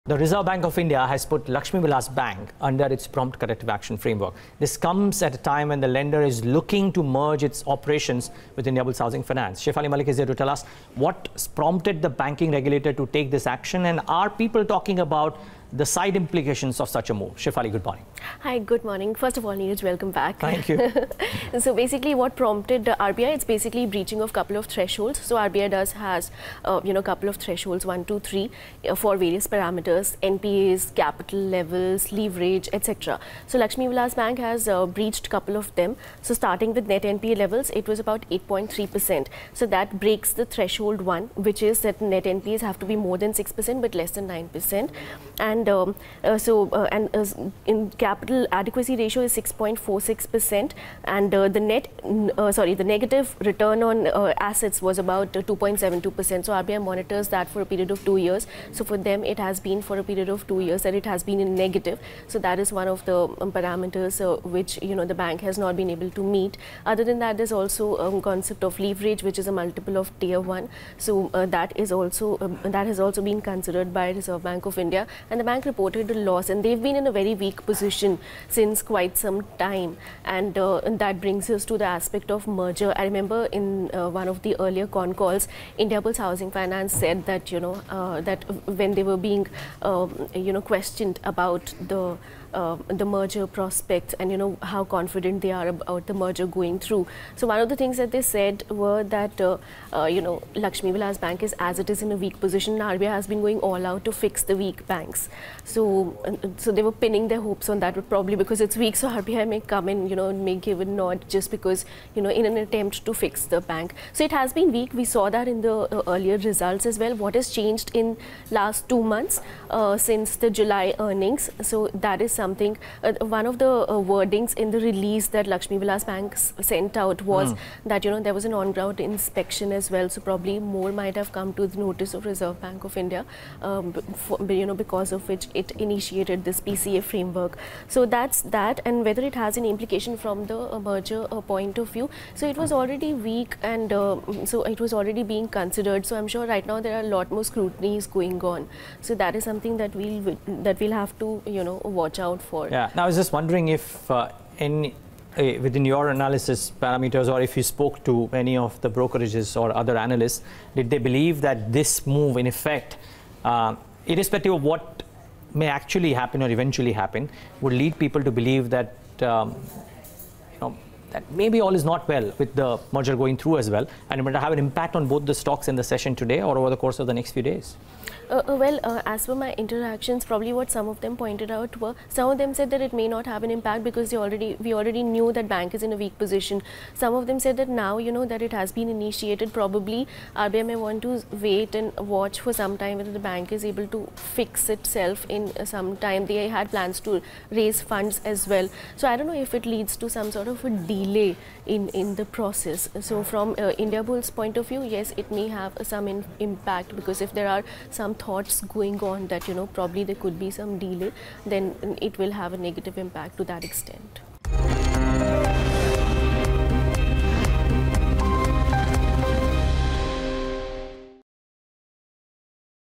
RBI Puts Lakshmi Vilas Bank Under PCA Framework
The Reserve Bank of India has decided to place private sector lender Lakshmi Vilas Bank Ltd. under its prompt corrective action framework, the lender informed stock exchanges in a notification on Saturday morning.